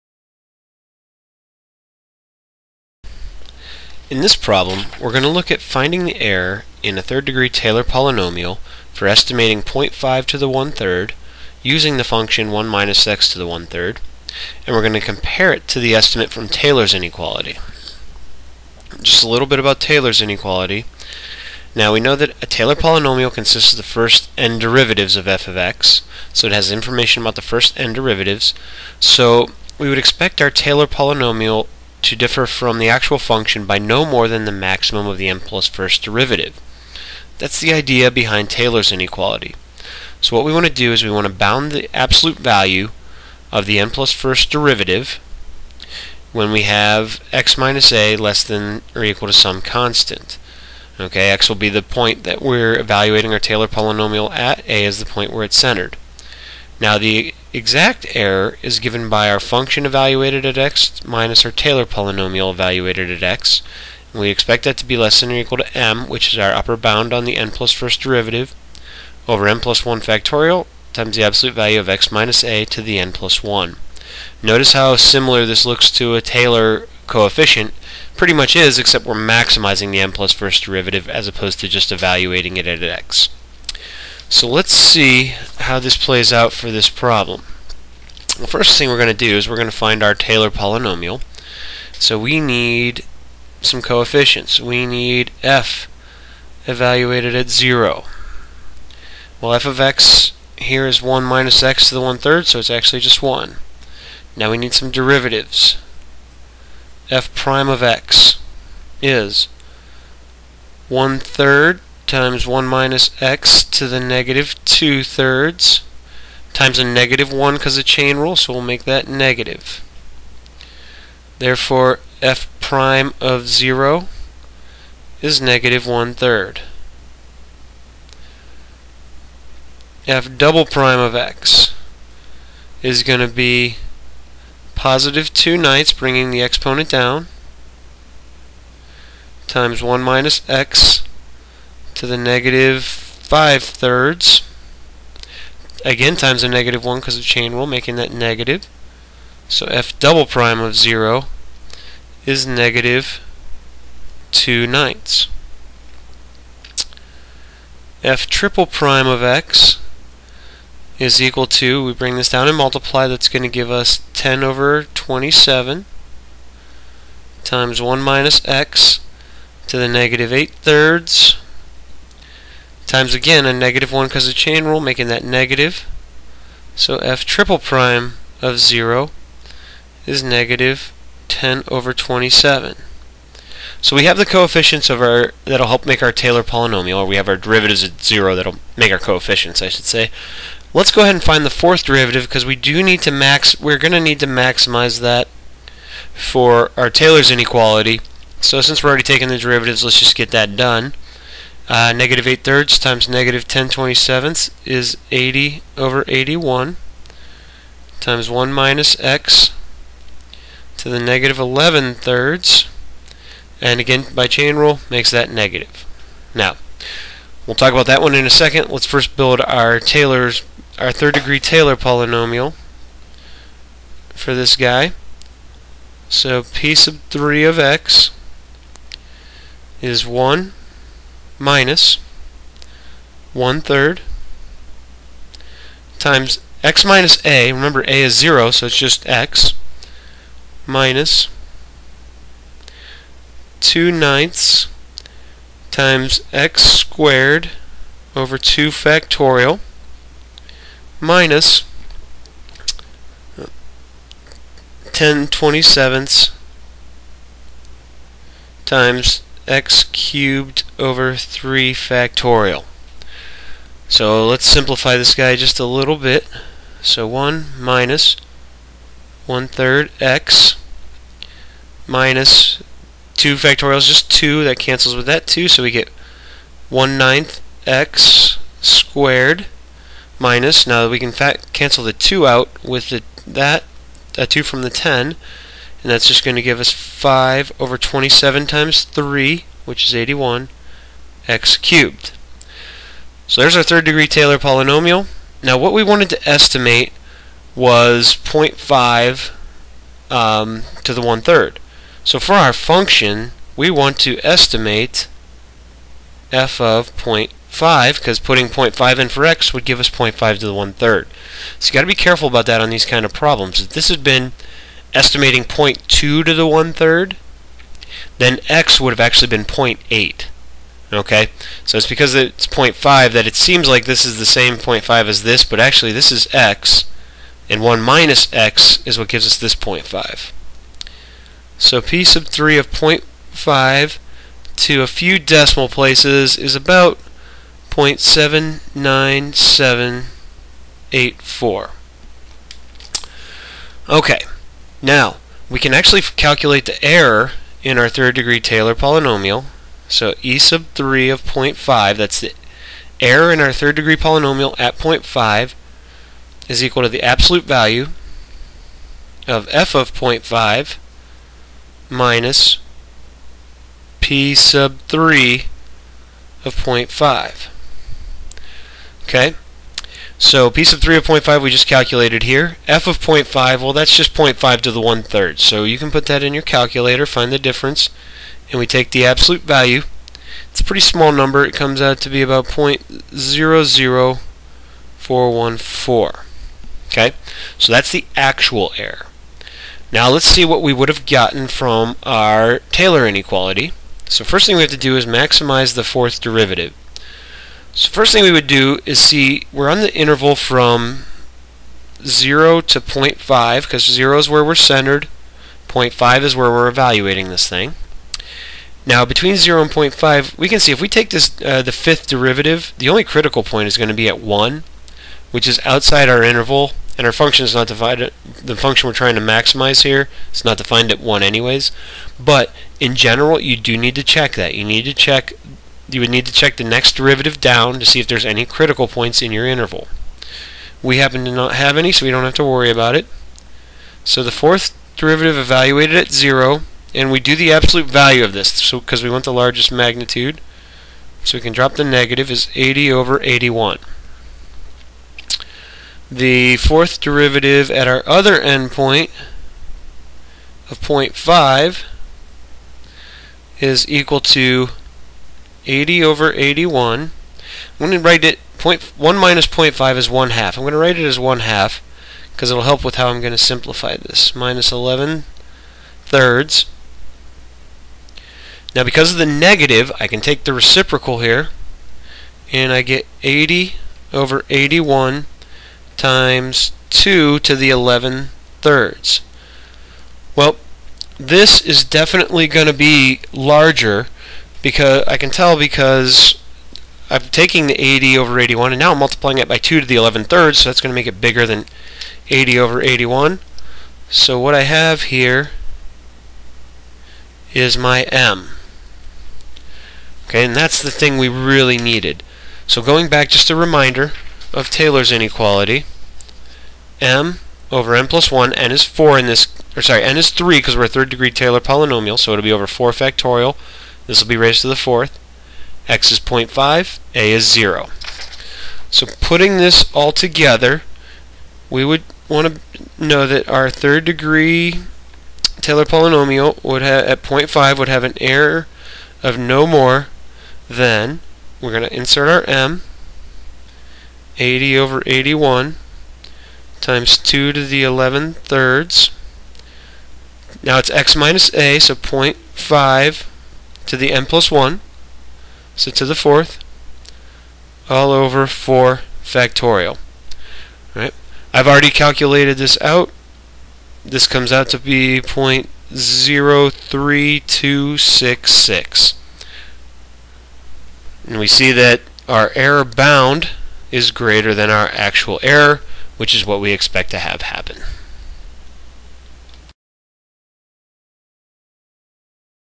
Taylor's Inequality - Estimating the Error in a 3rd Degree Taylor Polynomial
In this video we use Taylor's inequality to approximate the error in a 3rd degree taylor approximation. We then compare our approximate error with the actual error.